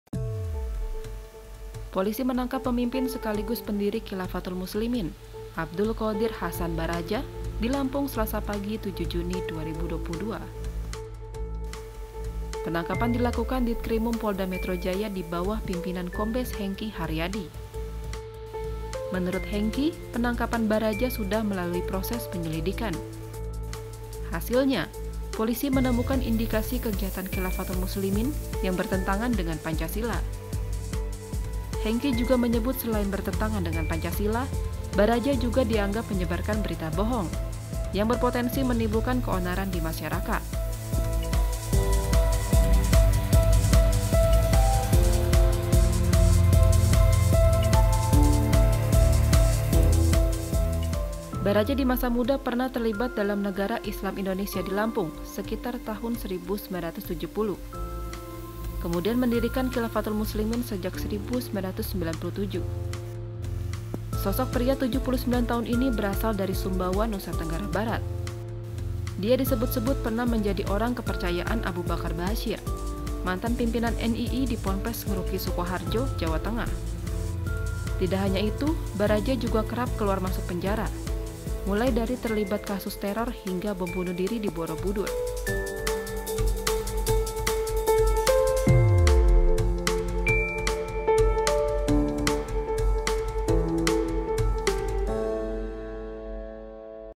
Tim Kombes Hengky Tangkap Pimpinan Khilafatul Muslimin yang Konvoi Motor di DKI
MERDEKA.COM - Polisi menangkap pemimpin, sekaligus pendiri Khilafatul Muslimin, Abdul Qadir Hasan Baraja, di Lampung, pada Selasa pagi 7 Juni 2022.
Penangkapan dilakukan Ditkrimum Polda Metro Jaya di bawah pimpinan Kombes Hengki Haryadi Menurut Hengky/ penangkapan Baraja sudah melalui proses penyelidikan